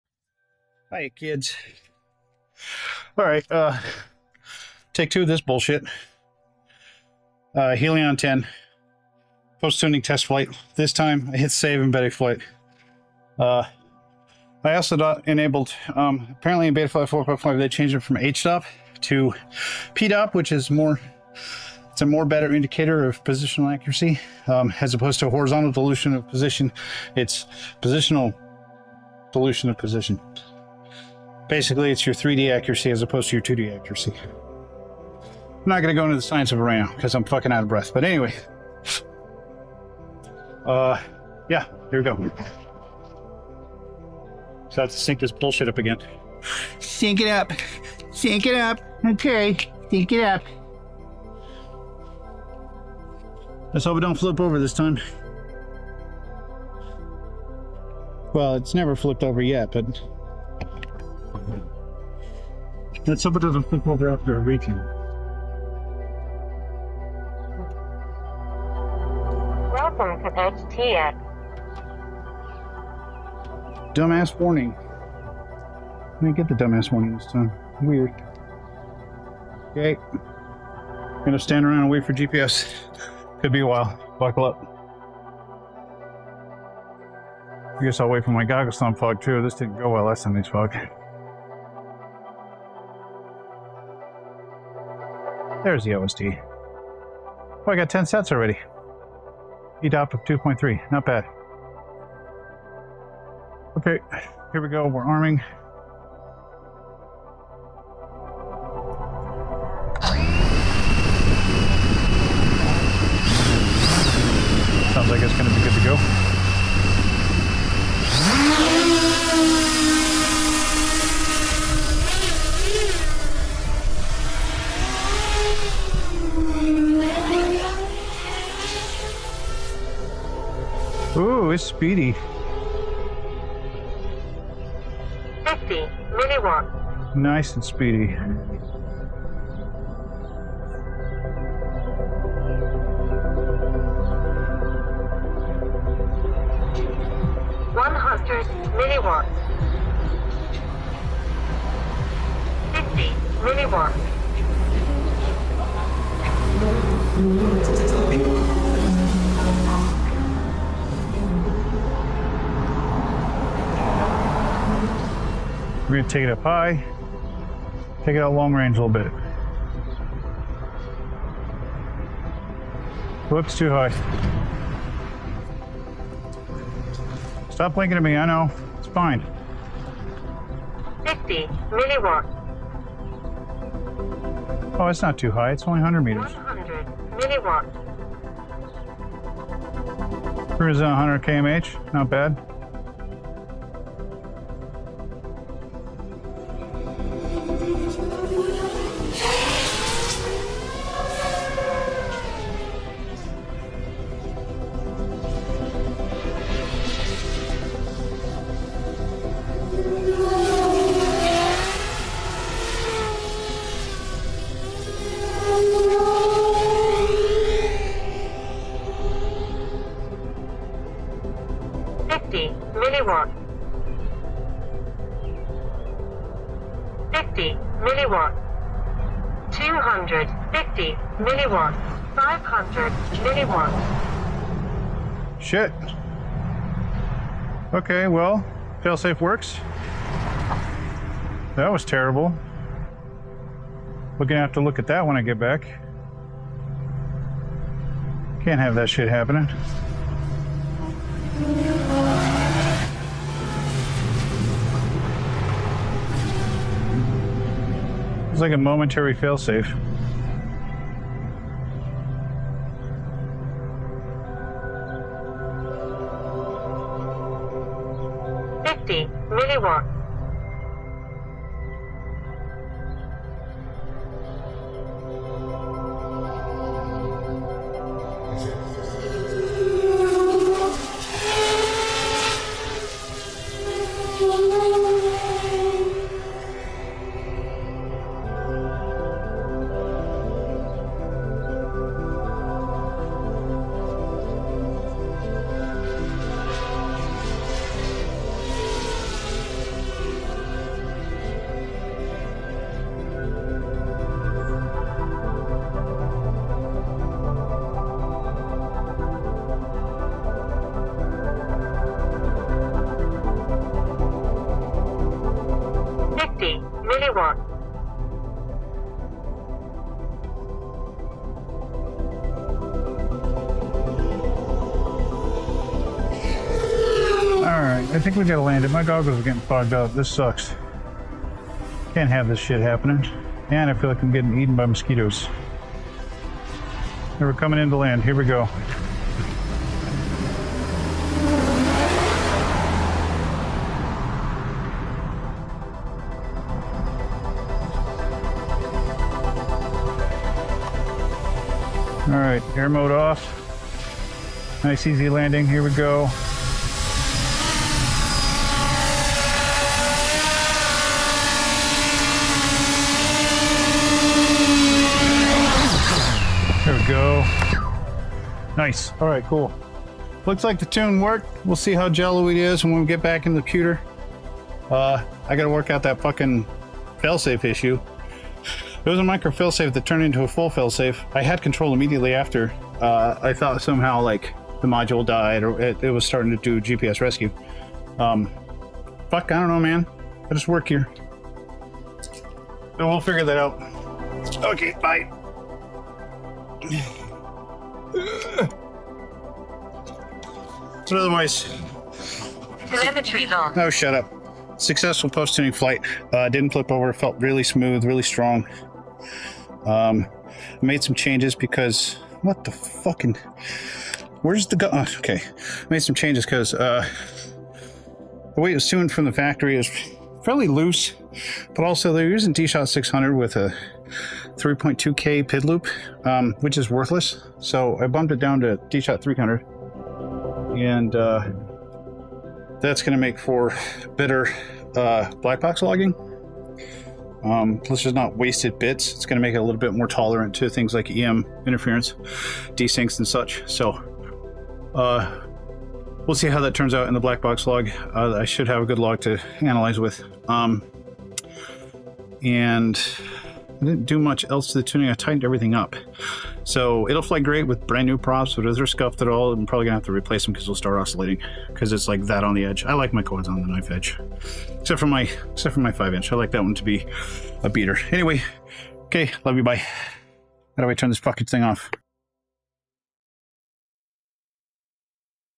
Post-Tuning Flight with the Helion 10: Bonus Failsafe
Took a quick cruise with the Helion 10 after upgrading to Betaflight 4.5 and a thorough tune-up, significantly tightening the tuning (no more jello). She's ready to rip on my trip! Except for the bonus failsafe due to the ELRS antenna being in a rather suboptimal position—which, to be fair, wasn't installed by me but by the folks at iFlight. A pile of Radiomaster RP4TD receivers are on the way to bring dual radio, quad channel, dual antenna RX goodness, ensuring I never lose connectivity with any of my drones.

* Betaflight 4.5 Upgrade: See the improvements in flight performance and stability after the latest firmware update.
* No More Jello: Enjoy the smooth, jello-free footage thanks to the improved tuning.
* Bonus Failsafe: A humorous look at the unexpected failsafe due to the antenna position, and the upcoming upgrade to Radiomaster RP4TD receivers for better connectivity.

Join me in this post-tuning flight and get ready for more reliable and exciting flights in the future.

- Share your experiences and connect with fellow drone enthusiasts.